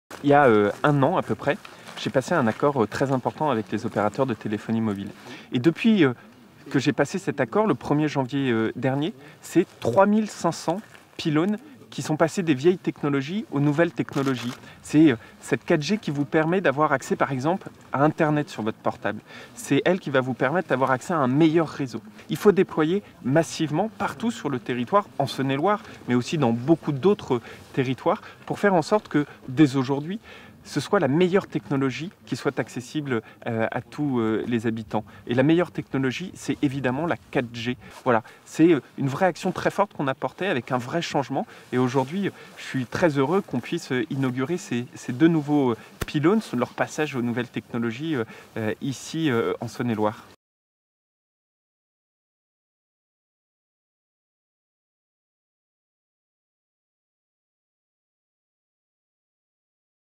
4G Saône-et-Loire - Interview de Julien Denormandie, Ministre à la Cohésion des territoires
Interview de Julien Denormandie, Ministre auprès de la ministre de la Cohésion des territoires et des Relations avec les collectivités territoriales, chargé de la Ville et du Logement, lors des inaugurations des deux premiers sites 4G multi-opérateurs d'accès à l'internet mobile dans le département de Saône-et-Loire (71) le 11 février 2019.